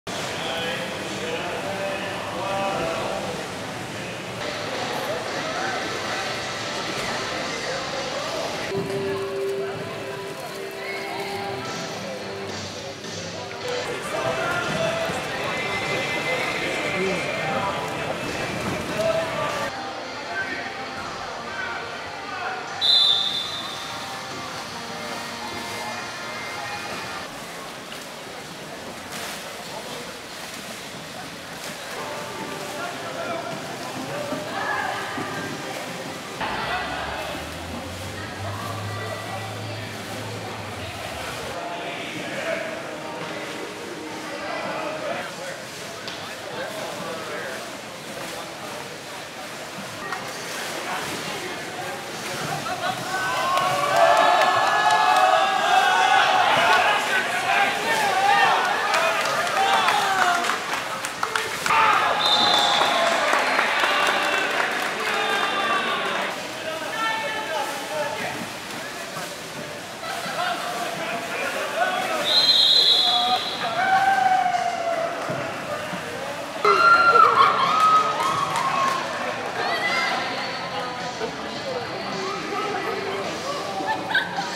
VEISHEA Battleship 2013 Highlights
One of the newest and most popular VEISHEA tournaments, battleship, took place at State Gym for the second year in a row. Here are highlights from Michael Kramer.